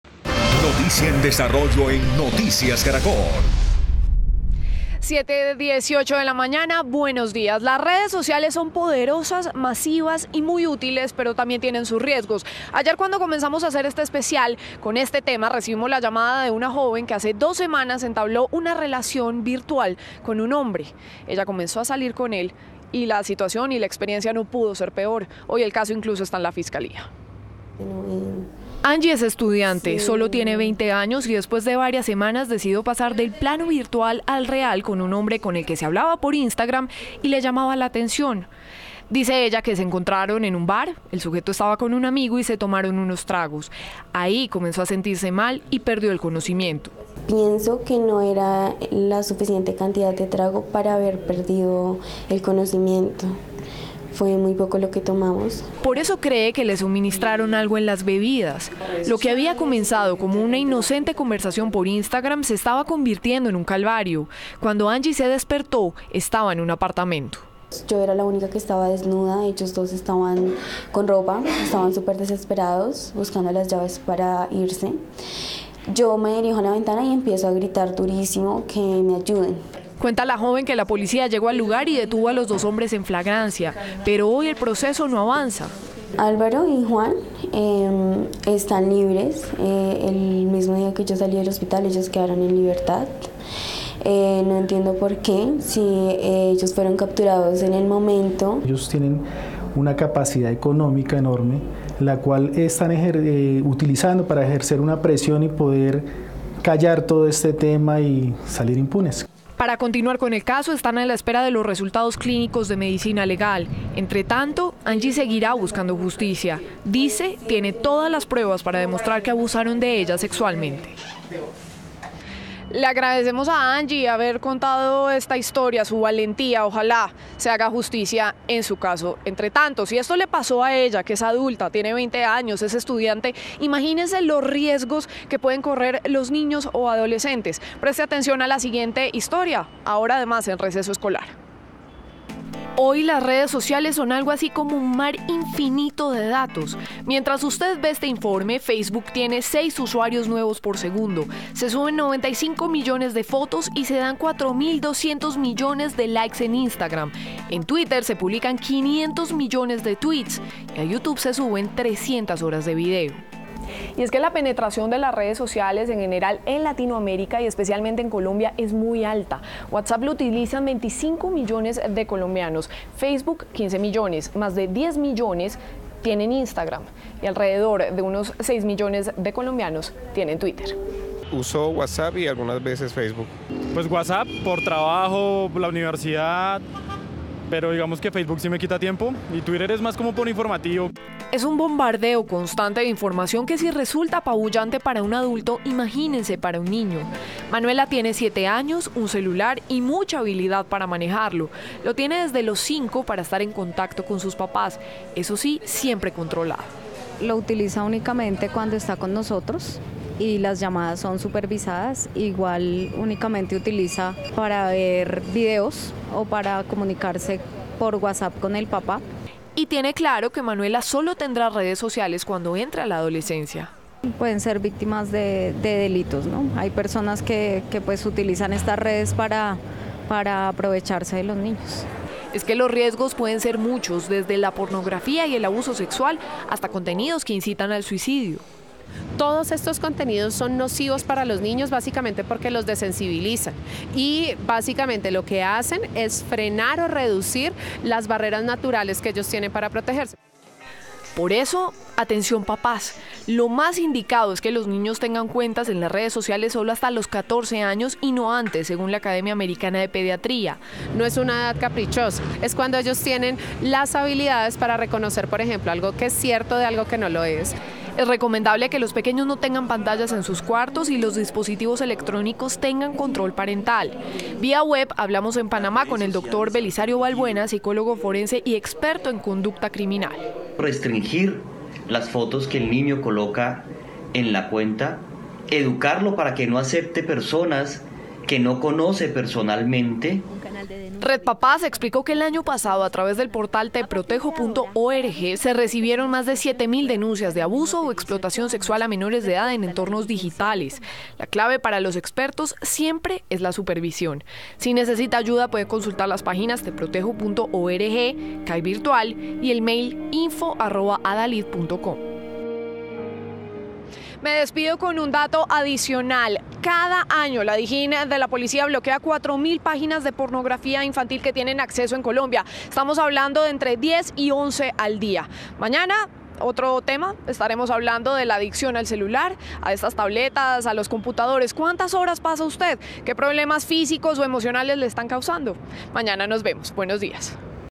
Redes sociales y menores de edad: consejos para que los papás no queden “off” | Noticias Caracol
Lo indicado es que abran cuentas después de los 14 años. Expertos aconsejan acompañarlos a conocer un mundo que tiene tanto de bueno como de malo.